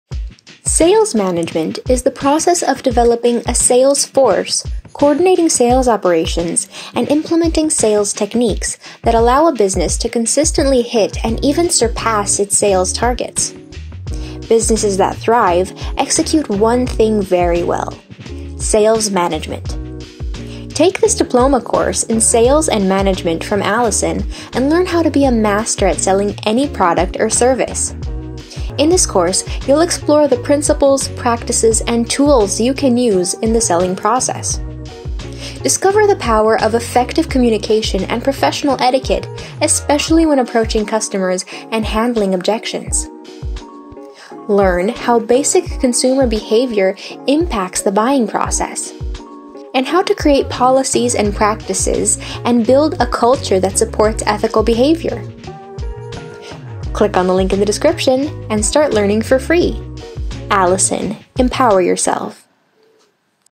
Diploma in Sales Management - Free Online Course with Certificate
In this video, you will learn about Alison’s Diploma in Sales Management course.

This free online sales management course will teach you about the principles, practices, and tools involved in the selling process. In this course, you will learn the importance of effective communication, business ethics, sales techniques, presentation skills, and how to understand and connect with your customers. In a business environment sales are all important, this course teaches you how to effectively sell your product or service.